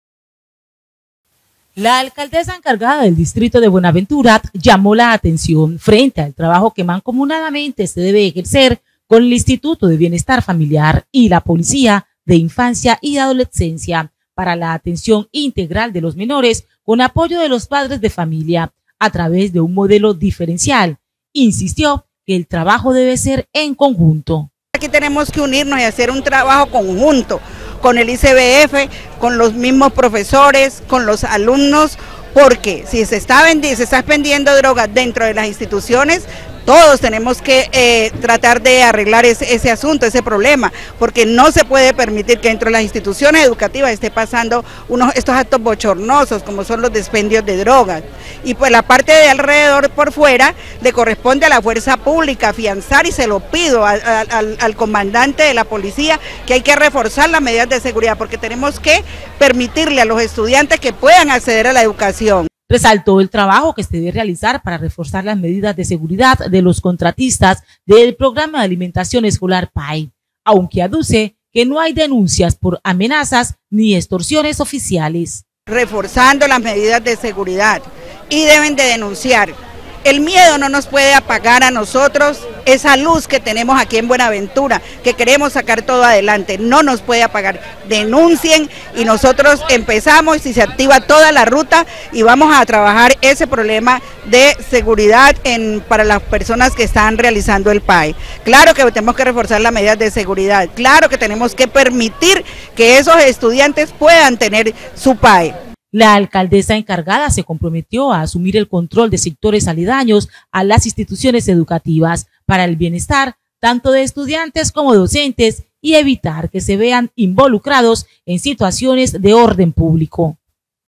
ALCALDESA Y ESTUDIANTES
La Alcaldesa encargada del Distrito de Buenaventura resaltó que la seguridad escolar es una responsabilidad compartida entre varias instituciones, con el respaldo activo de los padres de familia mediante un enfoque diferenciado. Además enfatizó en la necesidad de un trabajo conjunto y colaborativo.